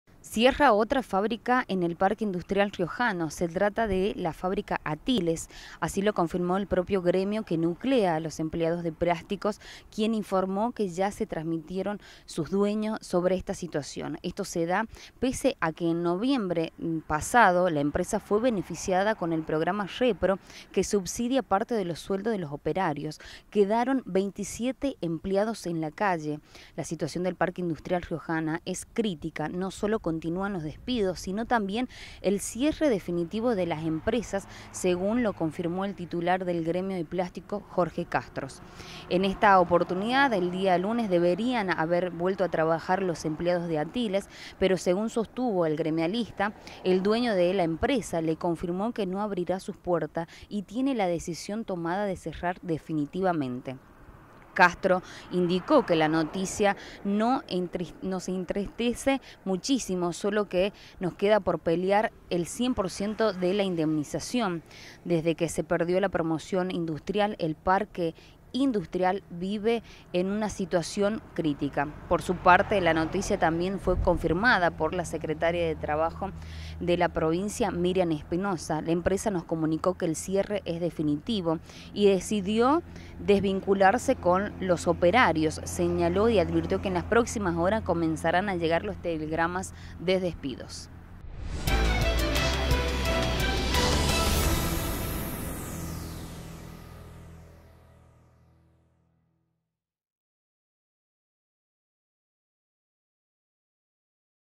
Cierre definitivo de la fábrica Atiles
Informe de Romina Zavaley
Lo anunciaron los propios empresarios. Hay 27 empleados en la calle.